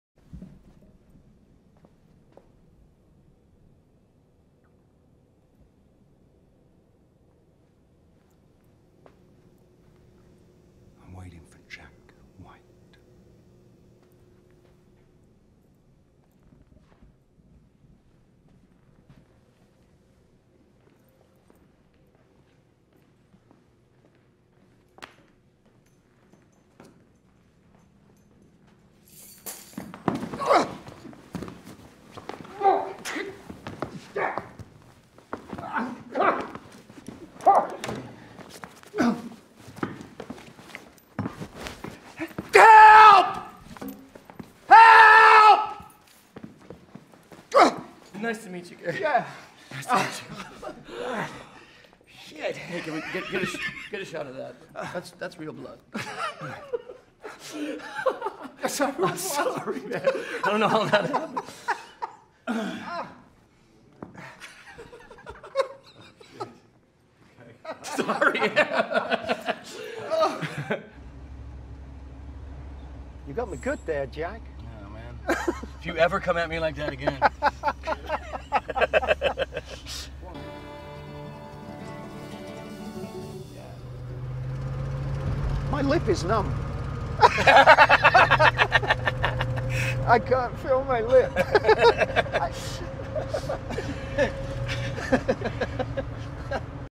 Jack White and Gary Oldman fight
Watch Jack White and Gary Oldman wrestle in Jack White's studio in this Amex UNSTAGED video.